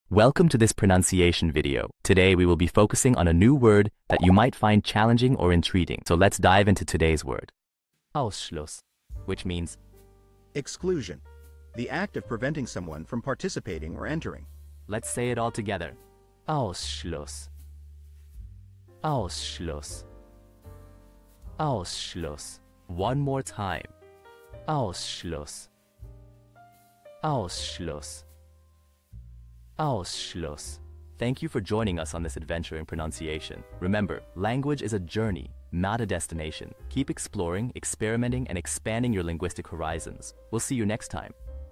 How to pronounce Ausschluß
Master the Pronunciation of 'Ausschluß which means exclusion' - which means : Exclusion;The Act of Preventing Someone from Participating or Entering. 📝 with @PronunciationGerman 🎓🗣️ - [Your Guide to German]

Learn to pronounce 'Ausschluß' perfectly in German with our easy and engaging tutorial by @PronunciationGerman! 📖📽️ In this comprehensive video, we provide you with native speaker examples 🗣️👂 and phonetic breakdowns 🎵 to ensure you get the pronunciation just right.

How to say Exclusion in German